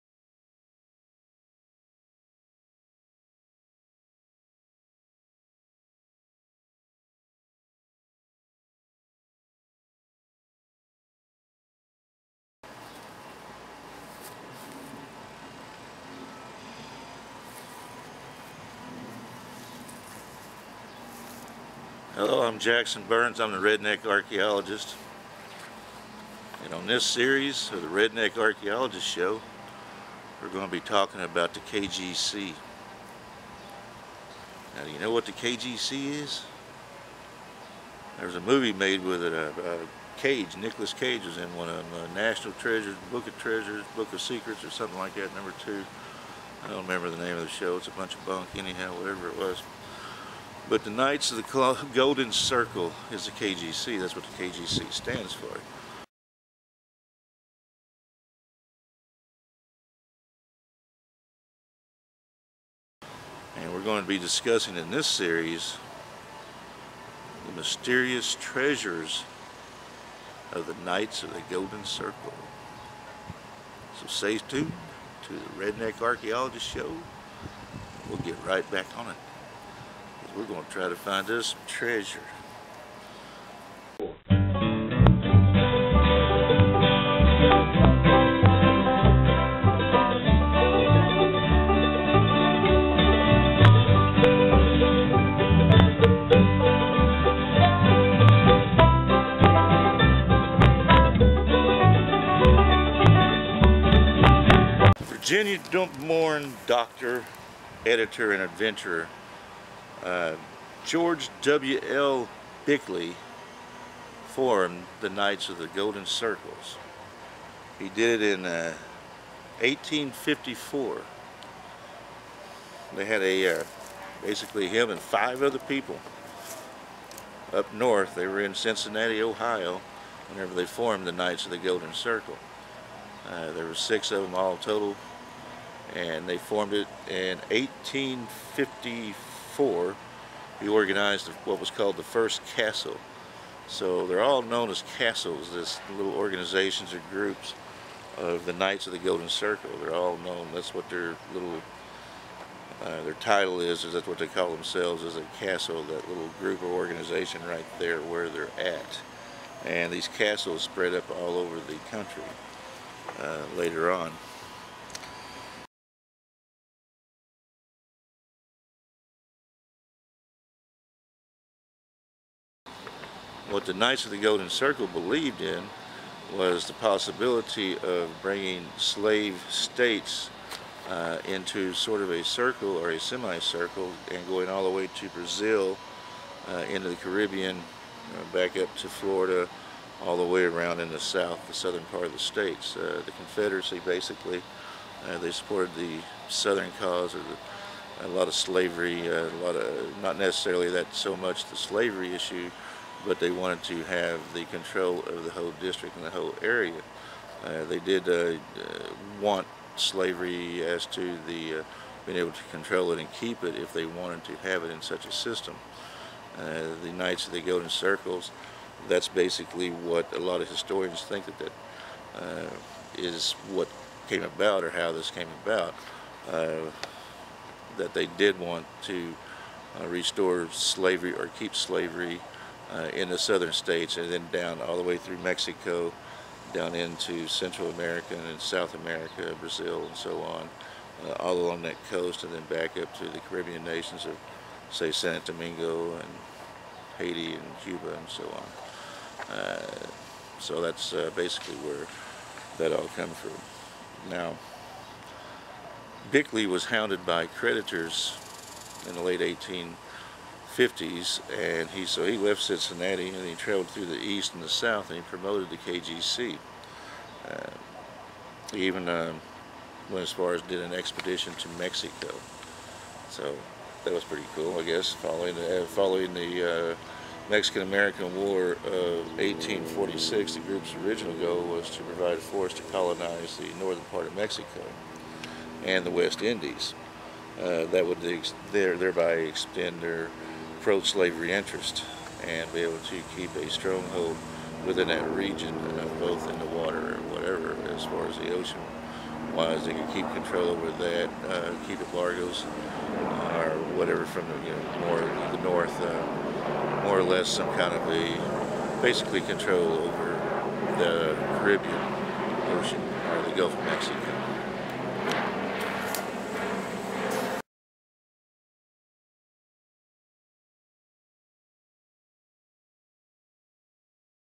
Mysteries of the Treasures of the Knights of the Golden Circle
Redneck Archaeologist, Jackson Burns, goes in search of the treasures of the secret organization Knights of the Golden Circle, Burns tracks the movements of the members across the US and locates caches of weapons and money hidden during and just after the Civil War